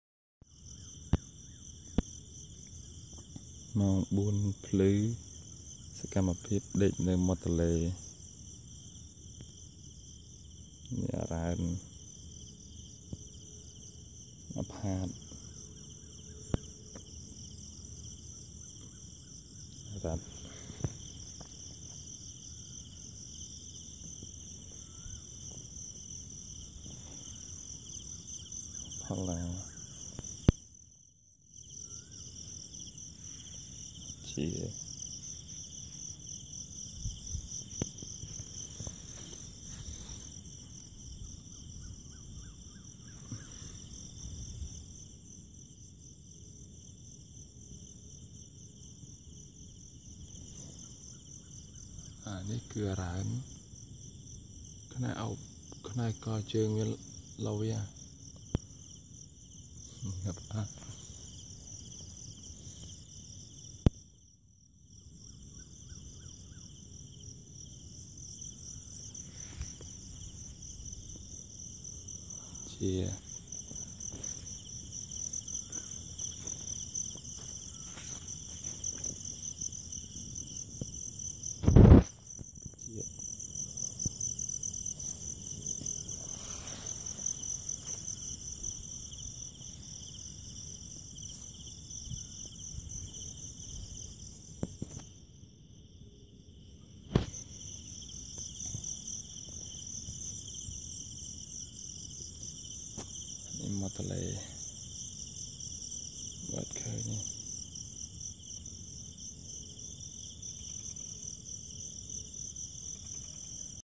one night at coconut school
After teaching them until 11pm, students and teacher sleep together. We are not only students and teacher but we are family. It is fun to study in coconut school teacher are like father, students are like daughters and sons. we love each other.